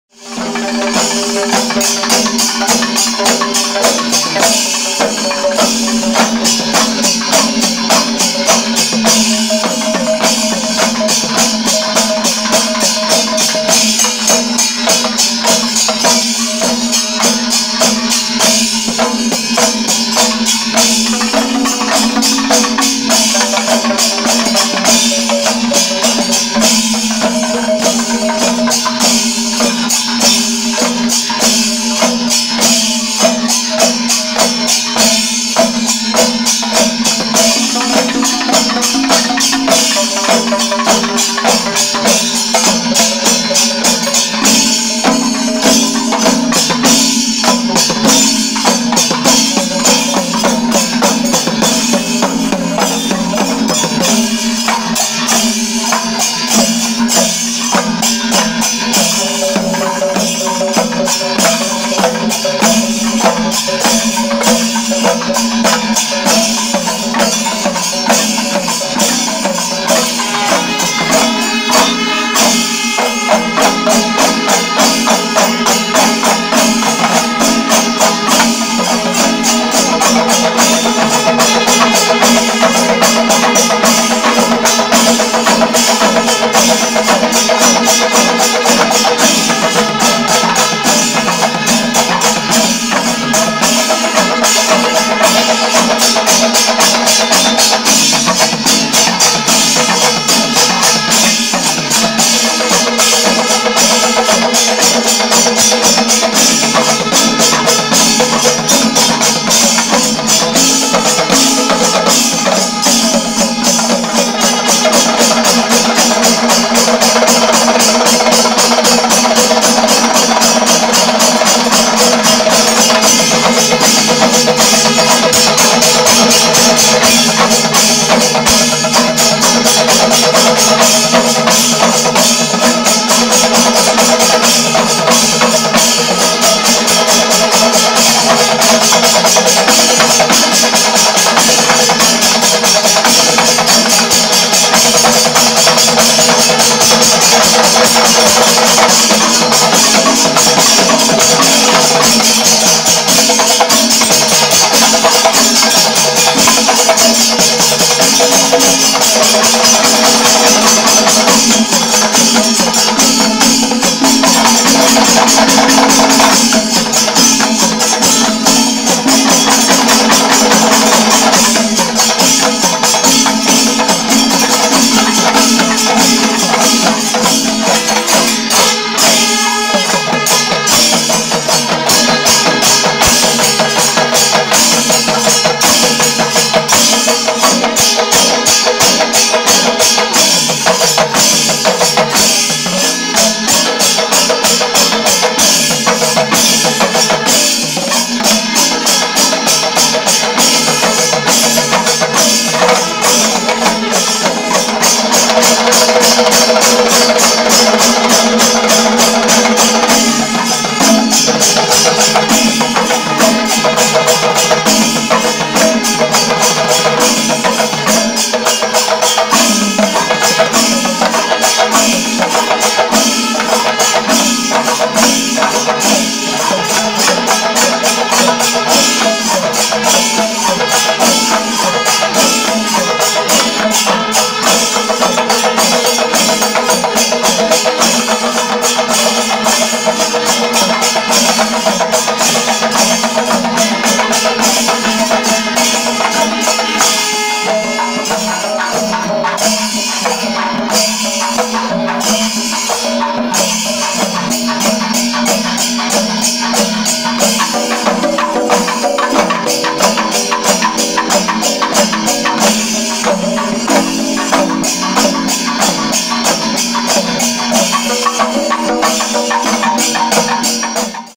Panchavadyam at Bhagavathy Sree Anna Poorneswari temple, Puthucode, Palakkad Dist, Kerala
Panchavdym at Bhagavathy Sree Anna Poorneswari temple, Puthucode, Palakkad Dist, Kerala